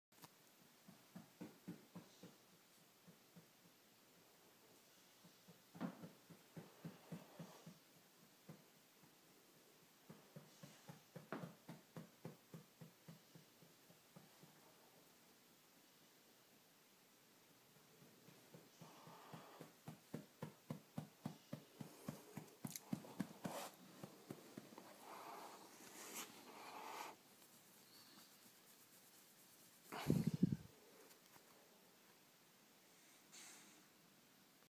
Beagle Tail on Auto Pilot
"Connick" the beagle has a tail with a mind of its own. He doesn't have to move an inch to show you he's excited to see you!